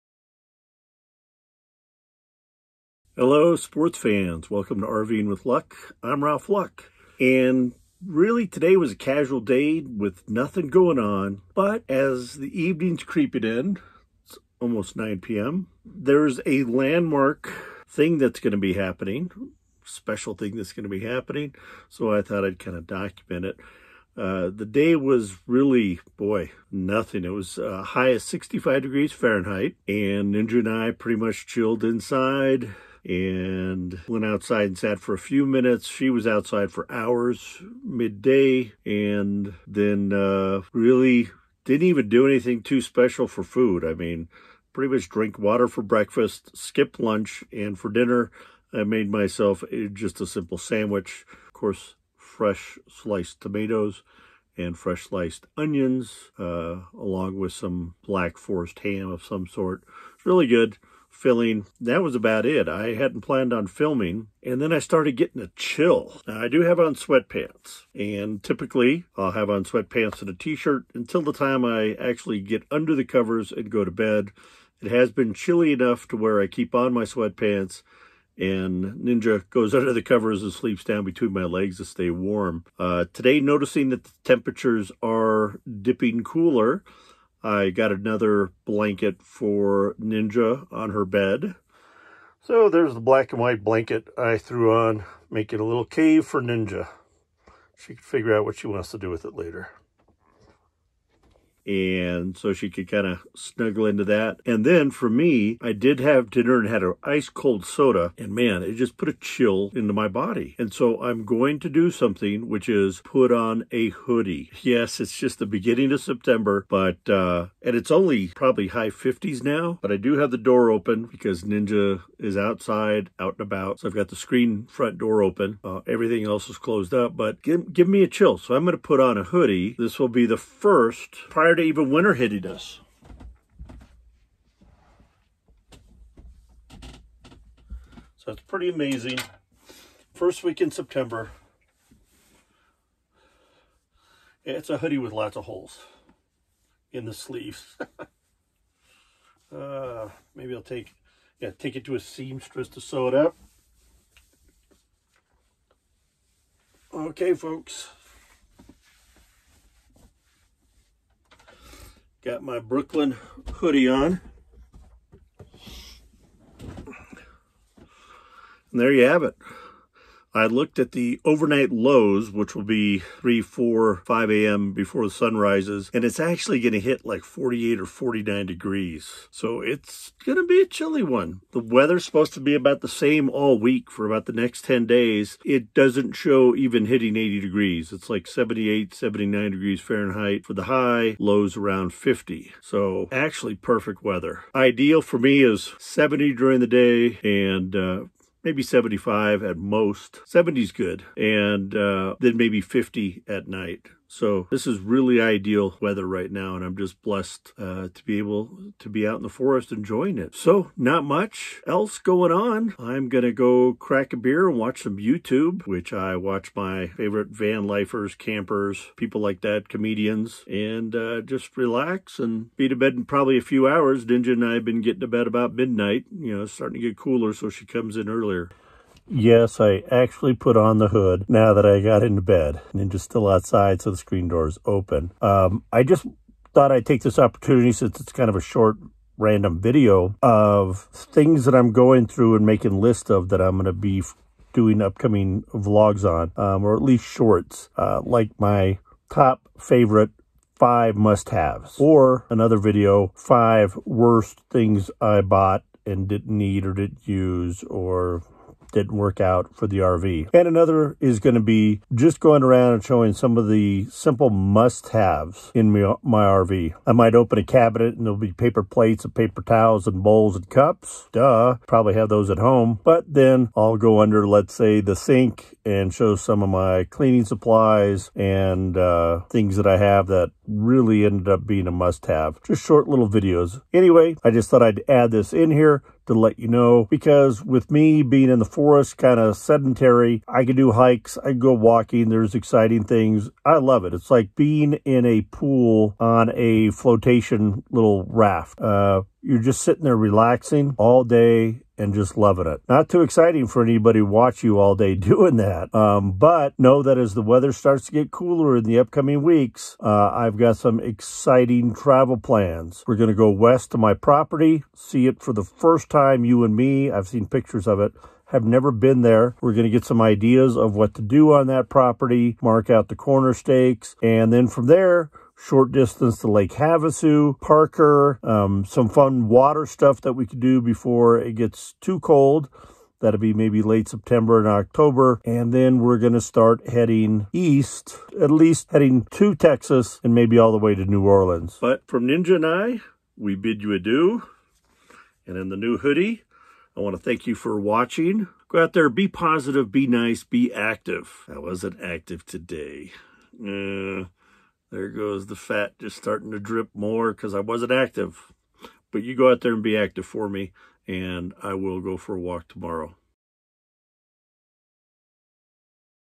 Chilly night//Full-time RV life
VLOG 31: A pretty lazy day, followed by a chilly night.
First time putting on a hoodie in several months.
Gave Ninja an extra blanket.

Be positive.
Be nice.
Be active.